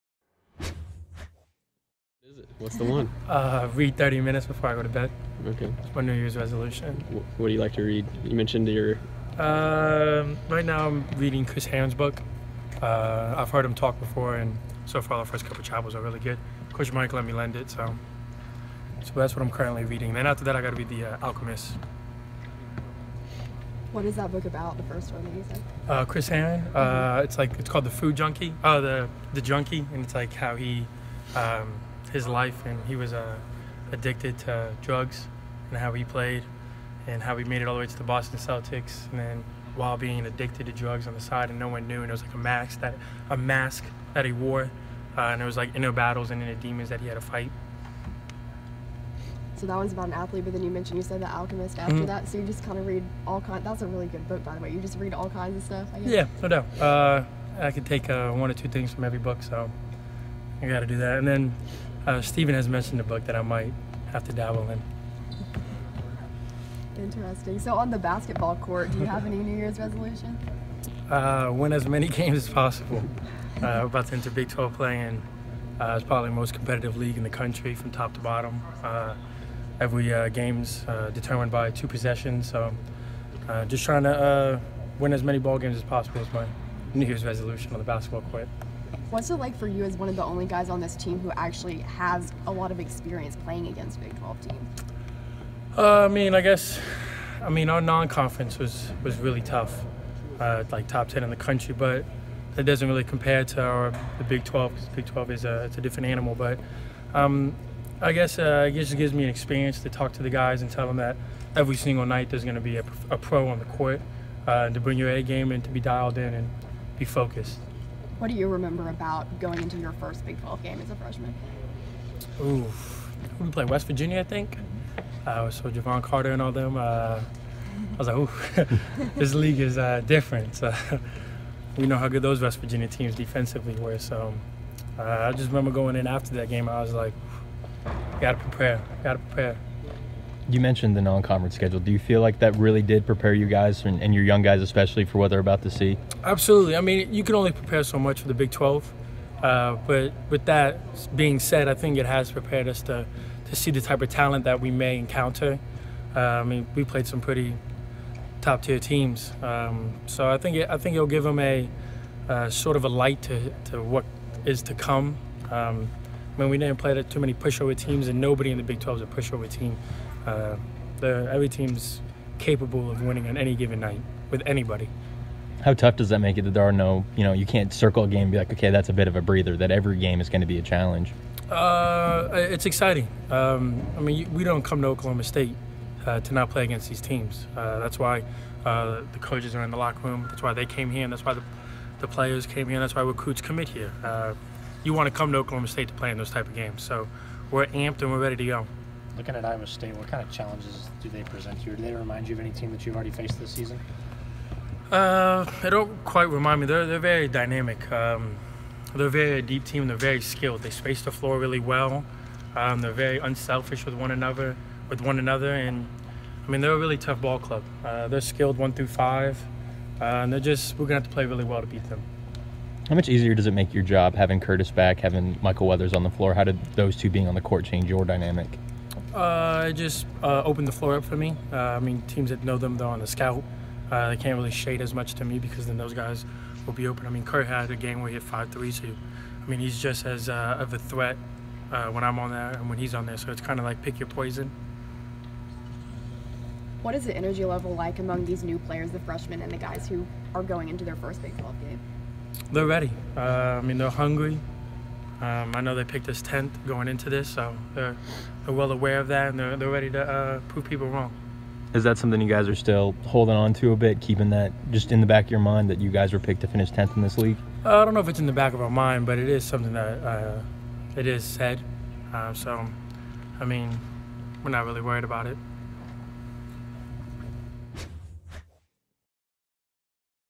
OSU Basketball - Thomas Dziagwa
Oklahoma State guard Thomas Dziagwa talks about his New Years resolutions and the Cowboys’ Big 12 opener against Iowa State.